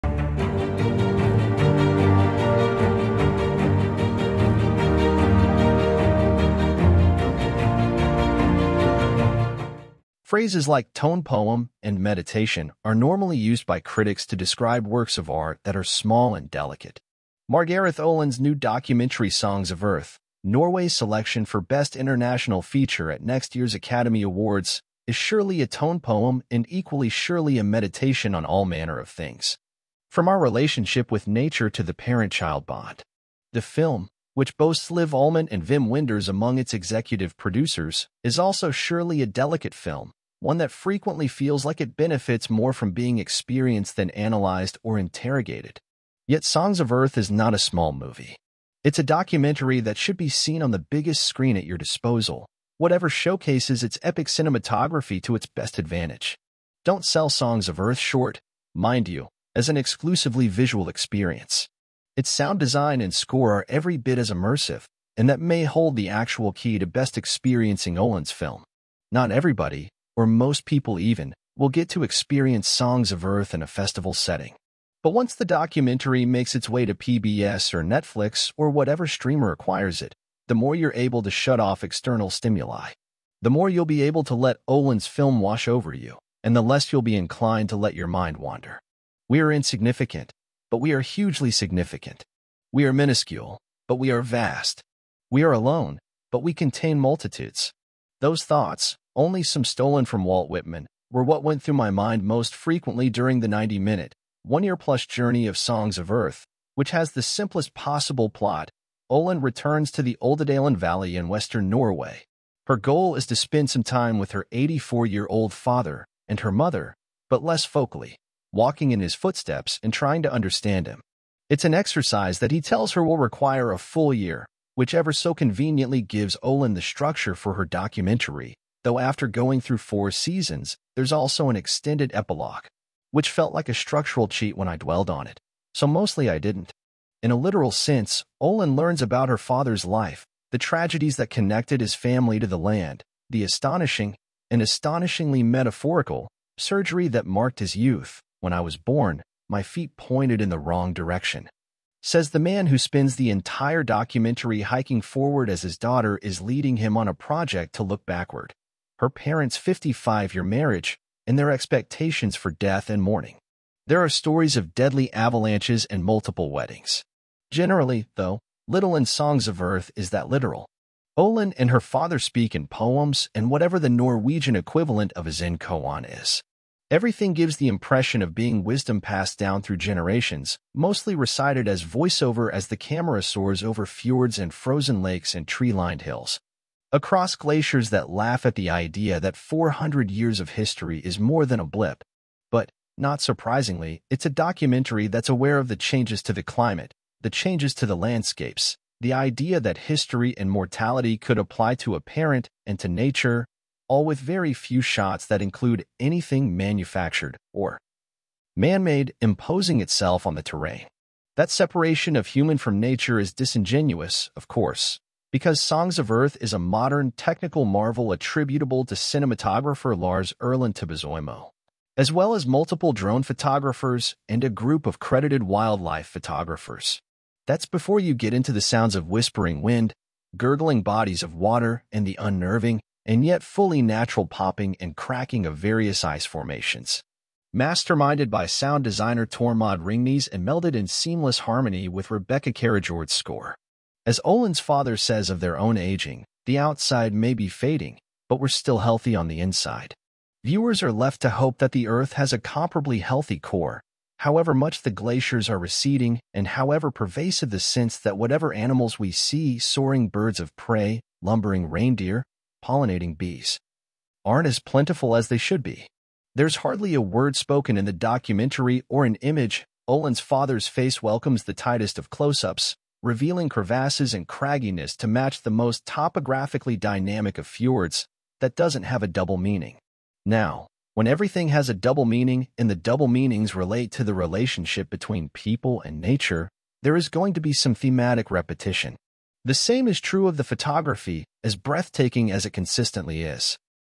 Songs of Earth’ Review: Delicate Norwegian Oscar Submission Meditates on Mortality and Nature
Phrases like “Tone poem” and “meditation” are normally used by critics to describe works of art that are small and delicate.

Margareth Olin’s new documentary Songs of Earth, Norway’s selection for best international feature at next year’s Academy Awards, is surely a tone poem and equally surely a meditation on all manner of things, from our relationship with nature to the parent-child bond. The film, which boasts Liv Ullmann and Wim Wenders among its executive producers, is also surely a delicate film, one that frequently feels like it benefits more from being experienced than analyzed or interrogated.Yet Songs of Earth is not a small movie. It’s a documentary that should be seen on the biggest screen at your disposal — whatever showcases its epic cinematography to its best advantage. Don’t sell Songs of Earth short, mind you, as an exclusively visual experience. Its sound design and score are every bit as immersive, and that may hold the actual key to best experiencing Olin’s film. Not everybody, or most people even, will get to experience Songs of Earth in a festival setting. But once the documentary makes its way to PBS or Netflix or whatever streamer acquires it, the more you’re able to shut off external stimuli, the more you’ll be able to let Olin’s film wash over you and the less you’ll be inclined to let your mind wander.